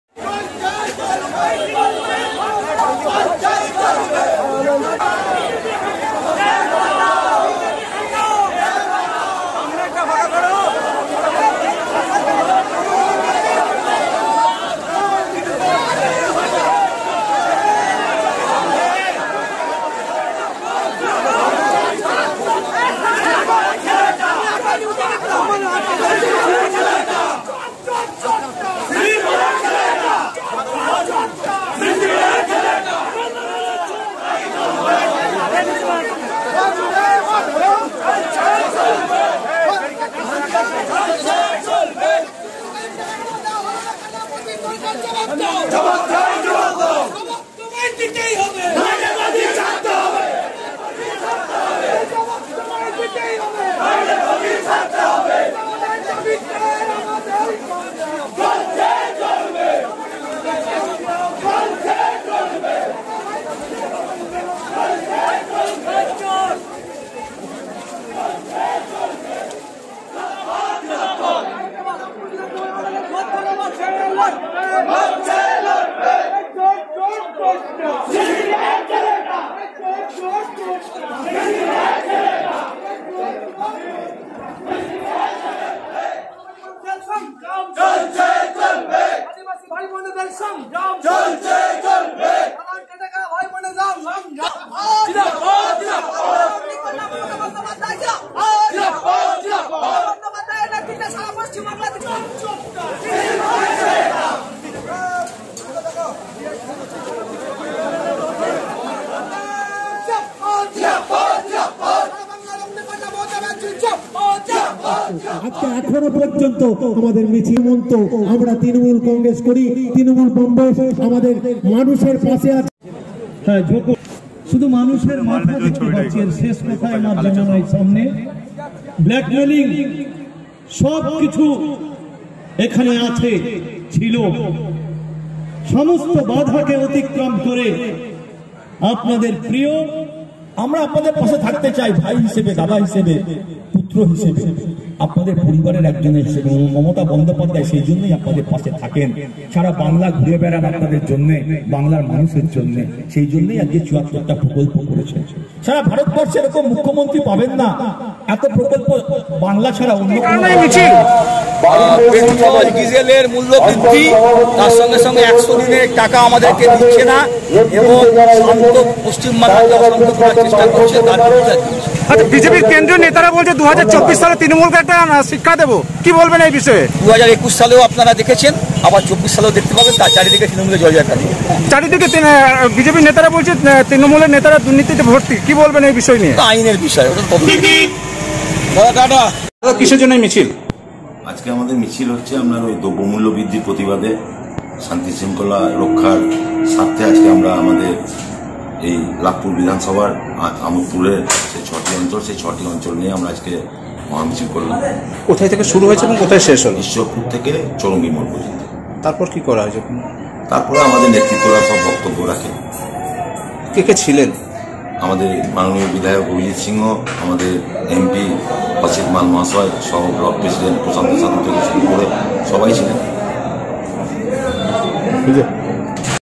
তৃণমূলের প্রতিবাদ মিছিল আমোদপুরে। সাঁইথিয়া নিউজ ডট কম
দ্রব্য, মূল্য বৃদ্ধি ও কেন্দ্রীয় সরকারের বিরুদ্ধে বিক্ষোভ মিছিল ও পথসভা করলো তৃণমূল। এদিন আমোদপুর অঞ্চলের কামারশাল মোড় থেকে প্রতিবাদ মিছিল শুরু হয়।শেষ হয় চৌরঙ্গী মোড়ে। সেখানেই পথসভা করে তৃণমূল।এই কর্মসূচীতে উপস্থিত ছিলেন সাংসদ অসিত মাল, বিধায়ক অভিজিৎ সিংহ, অঞ্চল সভাপতি রাজীব ভট্টাচার্য,